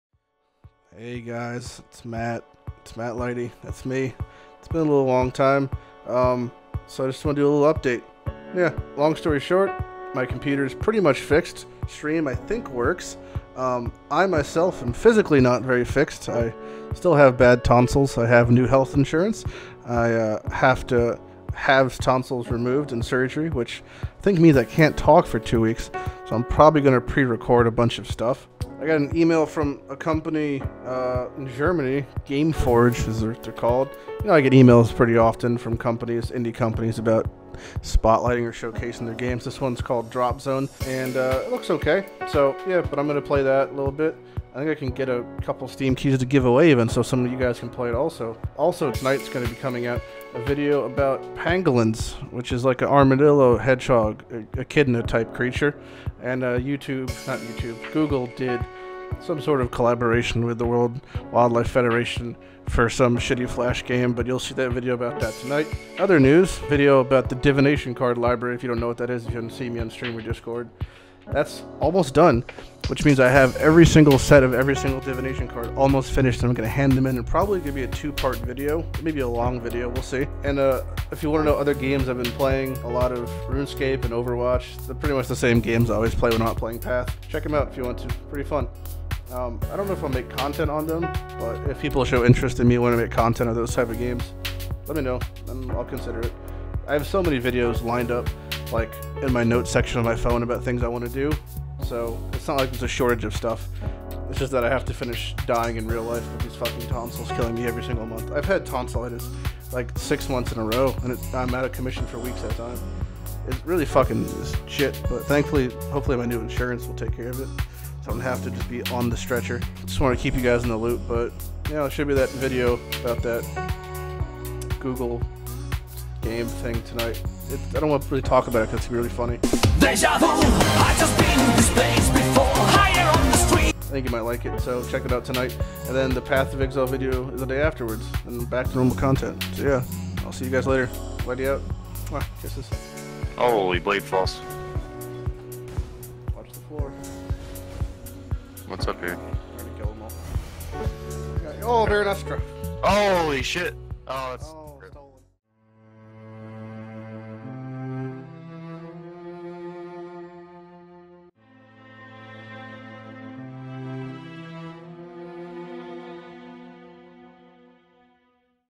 Monthly Update➥Still Alive, Divination Library, Pangolins, Sponsored Content & Other Games
つ ◕_◕ ༽つ Video Game Guides & Highlighty's™つ ◕_◕ ༽つ